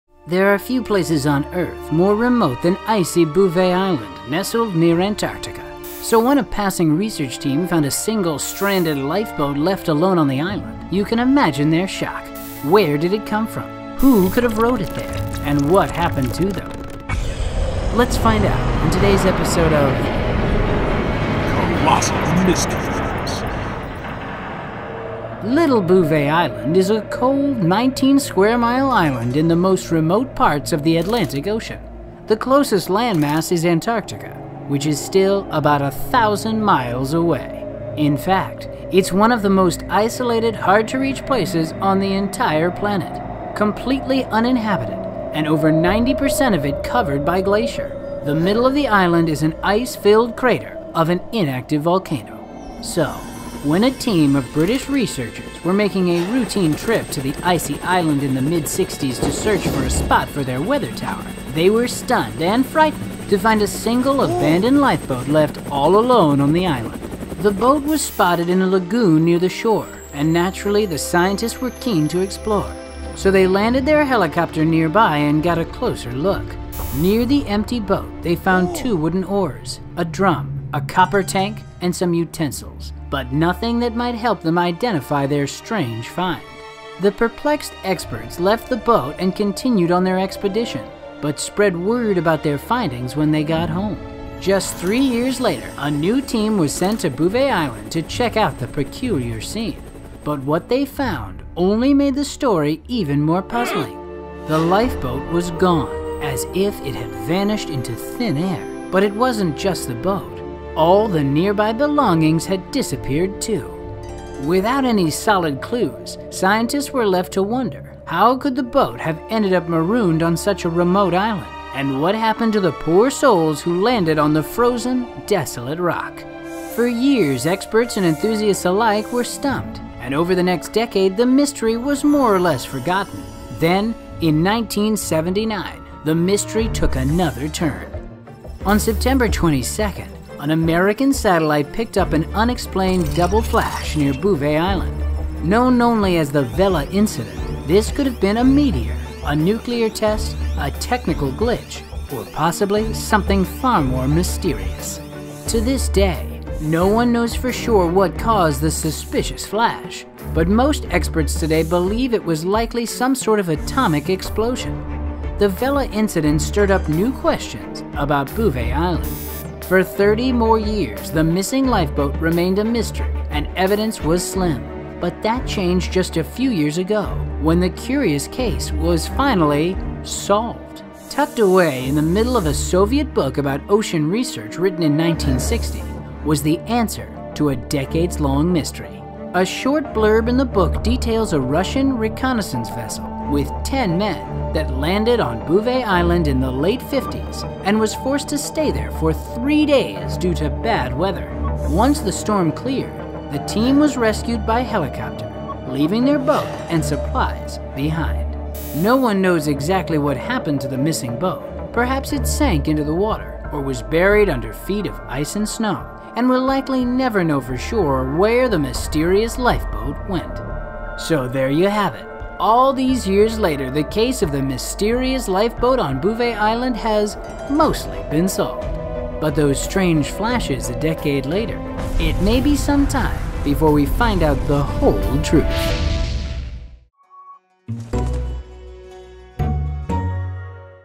LOST Mystery Boat in the Arctic | COLOSSAL MYSTERIES
There are few places on Earth more remote than icy Bouvet Island nestled near Antarctica. So, when a passing research team found a single, stranded lifeboat left alone on the island, you can imagine their shock! Where did it come from, who could have rowed it there, and what happened to them??

Welcome to Colossal Mysteries, the show that explores life's most puzzling mysteries!

→ Credits ←
Narrated by: Danny Tieger

Written & Directed by: Matt Levy
Edited & Animated by: Anton Makarau
Additional Animation by: Angela Chang